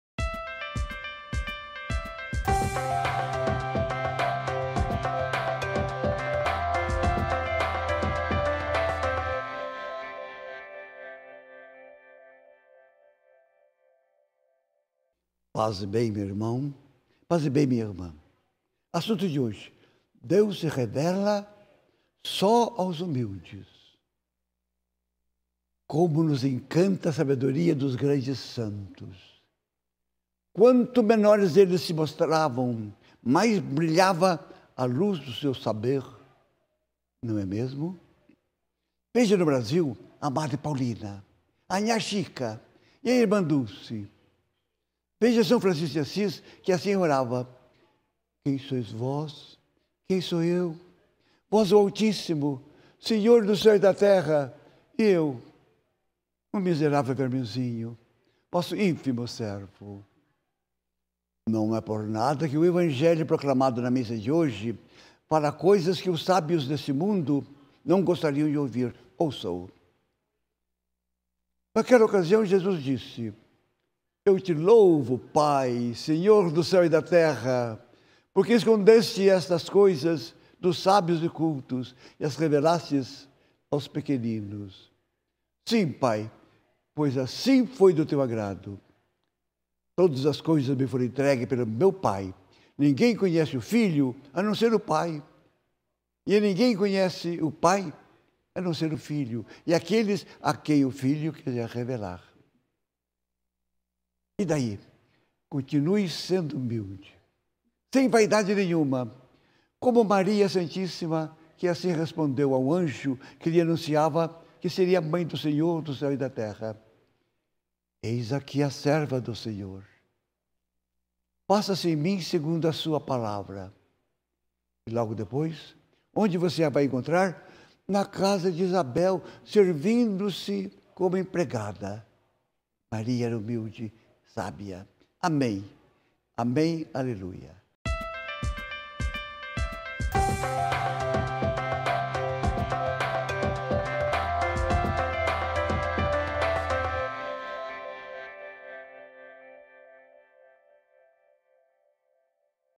Palavra da Hora - Deus se revela só aos humildes
Na série Palavra da Hora de hoje, citando São Francisco de Assis, Frei Filipinho ressalta que Deus se revela somente aos humildes. Acompanhe e compartilhe!